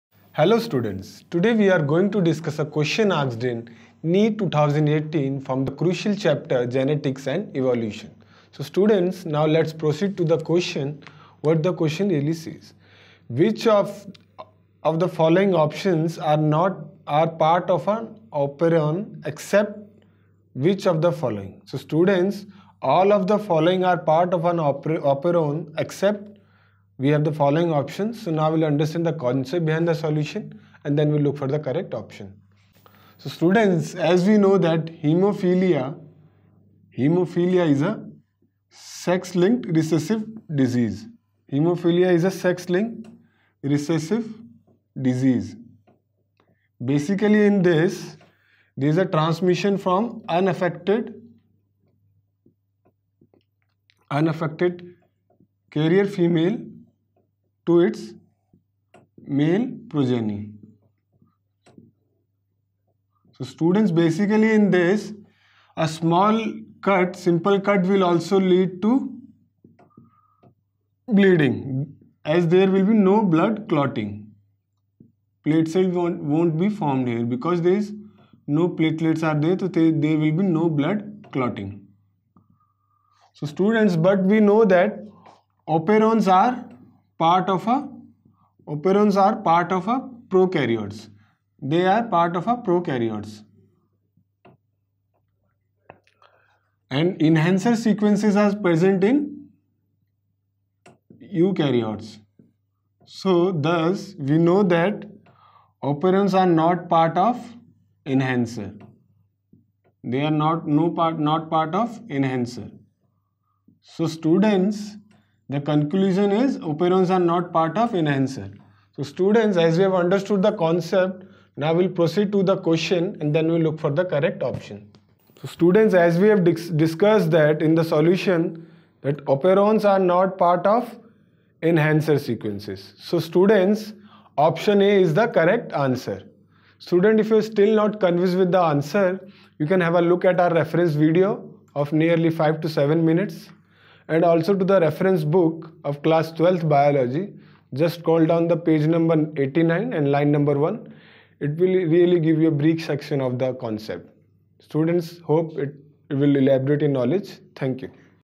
NEET 2018 Biology Question-All of the following are part of an operon except
Detailed Explanation:

All of the following are part of an operon except

    Option 1)

    an enhancer Option 2)

    structural genes Option 3)

    an operator Option 4)

    a promoter